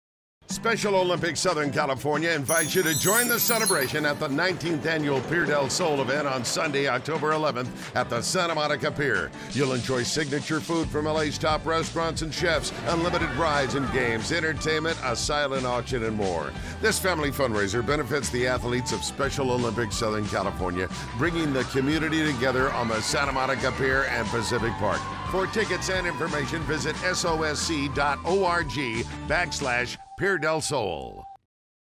Pier del Sol
The 19th annual Pier del Sol is a family fun event where guests can enjoy unlimited rides and games at Pacific Park, a VIP outdoor brunch with over 36 of Greater L.A.’s top restaurants and much more!